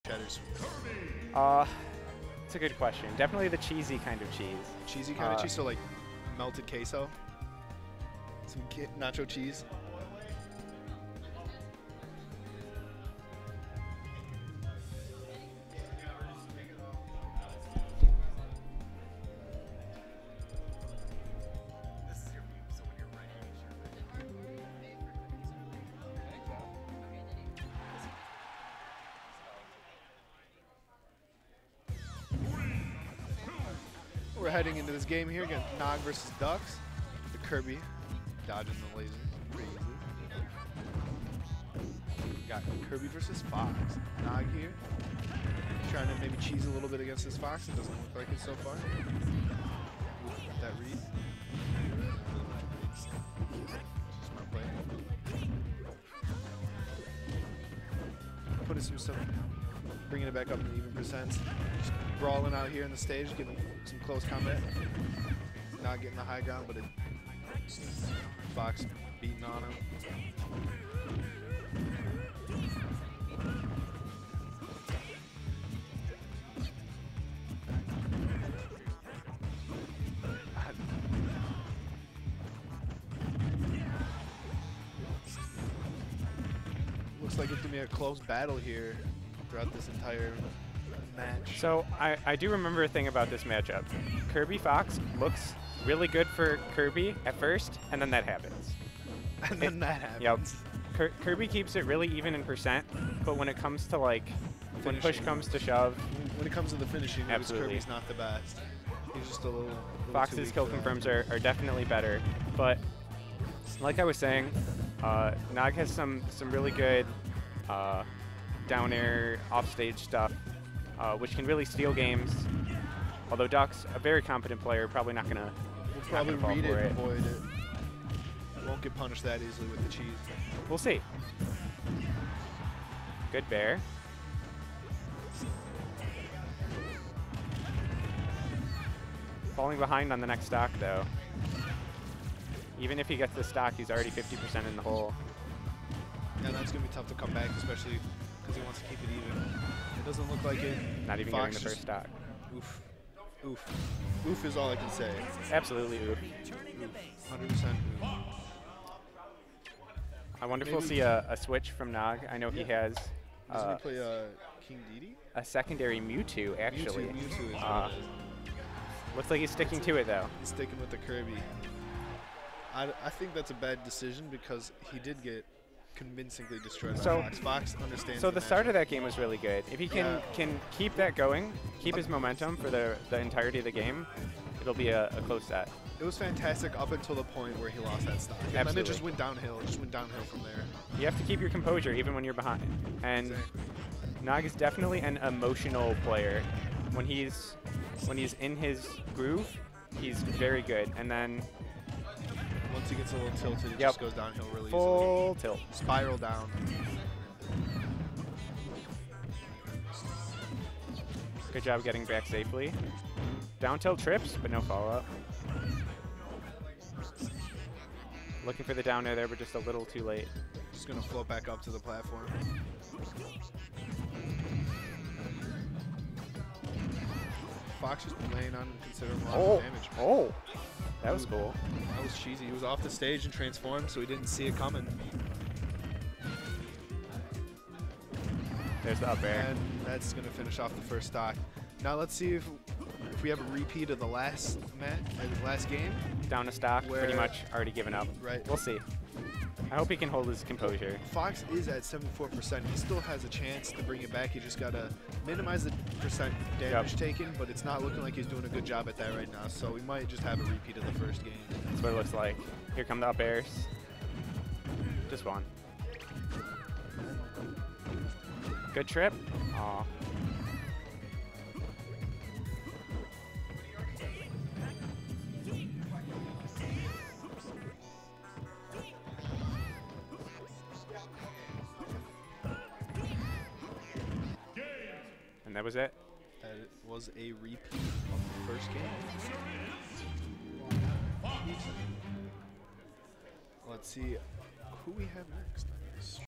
CoO IV: Dux (Fox) vs NoG (Kirby)
Chronicles of Olympus IV Singles Pool A2 WR1
4/22/18
Lansing, MI

Support GooshiGaming at:

Twitch.tv/GooshiGaming
Twitter.com/GooshiGaming
Facebook.com/GooshiGaming